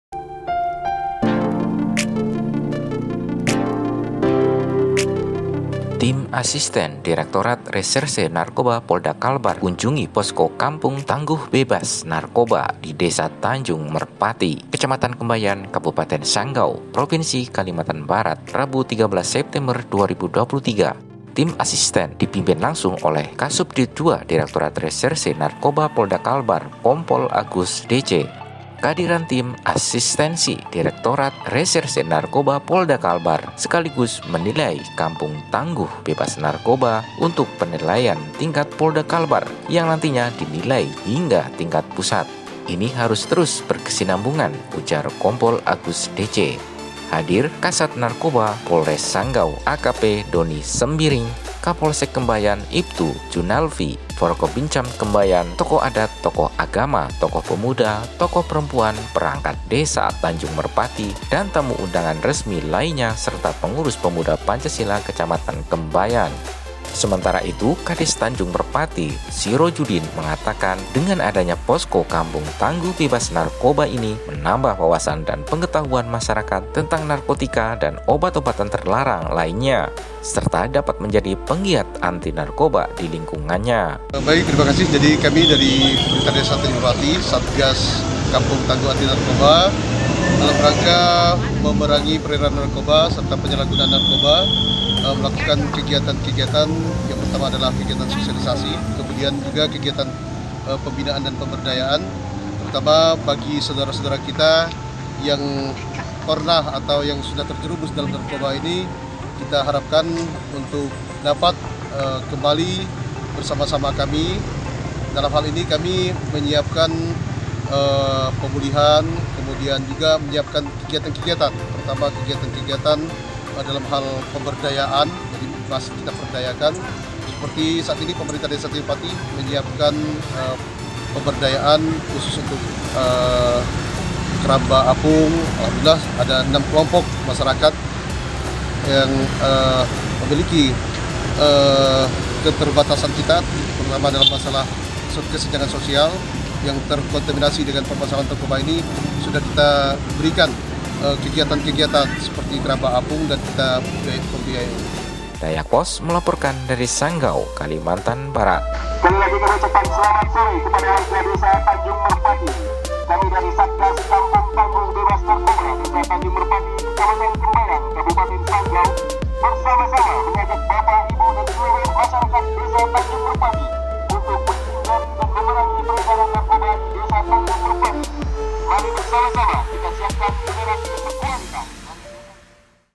Tim Polda Kalbar Tinjau Posko Kampung Tangguh Bebas Narkoba di Kembayan
SANGGAU - Tim Asistensi Direktorat Reserse Narkoba Polda Kalbar kunjungi Posko Kampung Tangguh Bebas Narkoba di Desa Tanjung Merpati, Kecamatan Kembayan, Kabupaten Sanggau, Provinsi Kalimantan Barat, Rabu 13 September 2023.

betangrayapost.com adalah media siber dan periklanan online terbesar di pulau Kalimantan